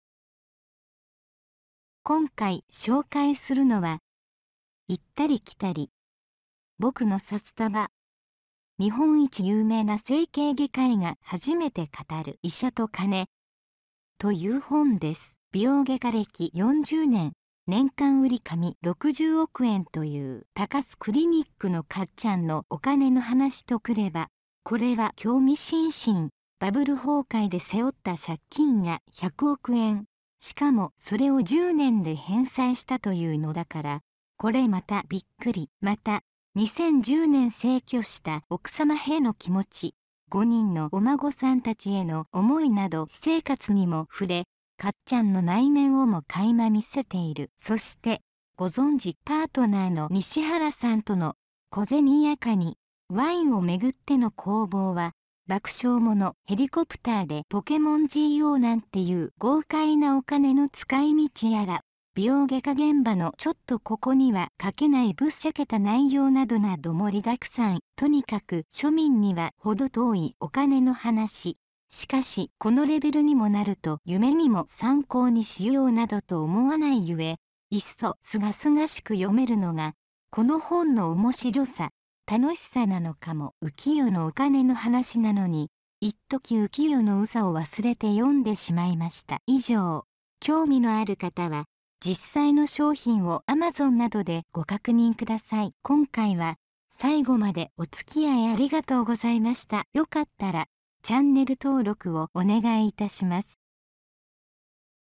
【紹介】行ったり来たり 僕の札束 日本一有名な整形外科医が初めて語る医者とカネ （高須 克弥）
今回紹介するのは、　行ったり来たり 僕の札束 日本一有名な整形外科医が初めて語る医者とカネ　という本です。

美容外科歴４０年、年間売上６０億円という高須クリニックのかっちゃんのおカネの話と来れば、これは興味深々！
バブル崩壊で背負った借金が１００億円、しかもそれを１０年で返済したというのだから、これまたビックリ。
また、２０１０年逝去した奥様ヘの気持ち、５人のお孫さん達への思いなど私生活にも触れ、かっちゃんの内面をも垣間見せている。
そして、ご存知パートナーの西原さんとの、小銭やカニ、ワインを巡っての攻防は、爆笑もの！
ヘリコプターでポケモンＧＯなんていう豪快なお金の使い道やら、美容外科現場のちょっとここには書けないぶっちゃけた内容などなど盛り沢山。
とにかく庶民には程遠いおカネの話、しかしこのレベルにもなると夢にも参考にしようなどと思わない故、いっそ清々しく読めるのが、この本の面白さ、楽しさなのかも・・。浮世のおカネの話なのに、いっとき浮世の憂さを忘れて読んでしまいました。